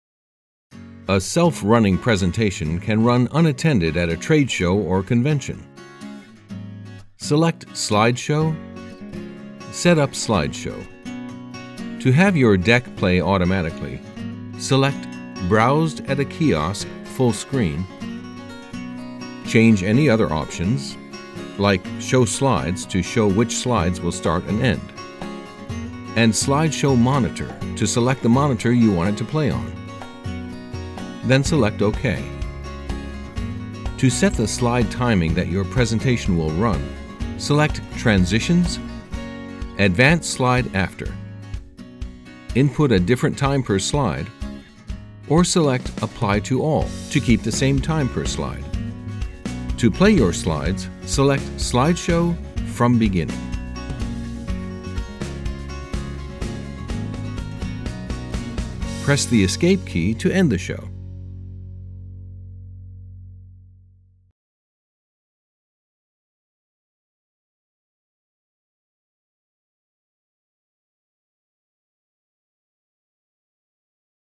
Auto-play a PowerPoint presentation | Microsoft
With Microsoft PowerPoint, you can use a self-running presentation to run unattended in a booth or kiosk at a trade show or convention, or you can save it as a video and send it to a client. Learn how to automatically advance slides in a PowerPoint slideshow in this video tutorial from Microsoft.

1. On the Slide Show tab, click Set Up Slide Show.

2. Under Show type, pick one of the following:

- To allow the people watching your slide show to have control over when they advance the slides, select Presented by a speaker (full screen).

- To present your slide show in a window, where control over advancing the slides is not available to the people watching, select Browsed by an individual (window).

- To loop your slide show until the people watching press Esc, select Browsed at a kiosk (full screen).